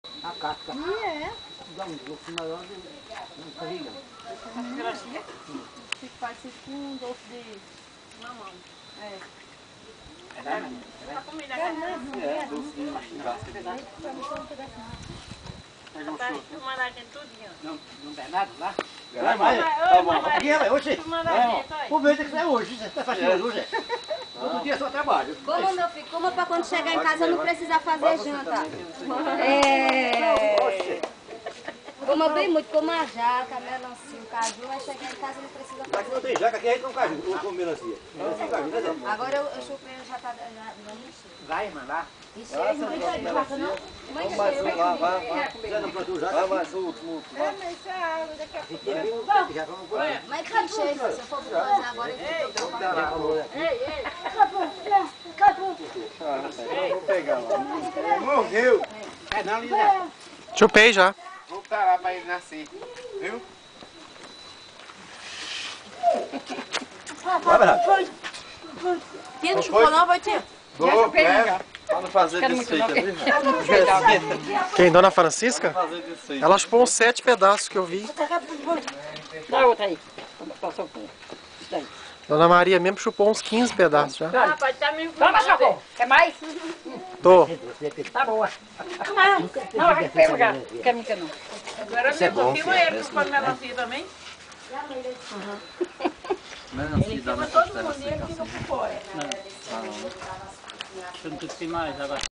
Quanta saudade!
No sítio do Zé Júlio. Pedro e outras pessoas da congregação. Dona Maria, Milca, Seu Zezinho e eu.